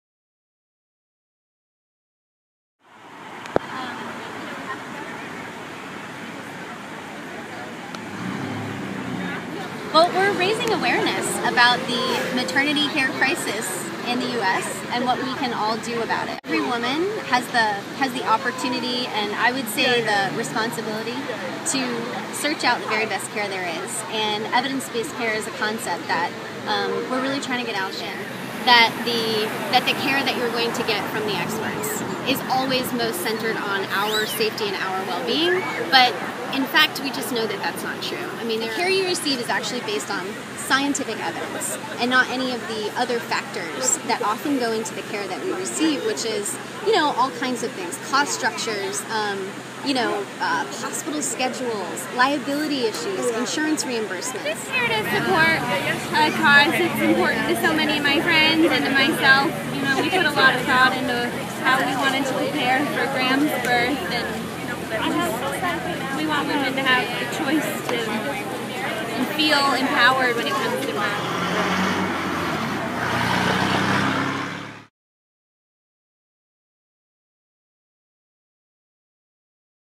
Lexington's Rally to Improve Birth
Rally, fair focus on women's options during pregnancy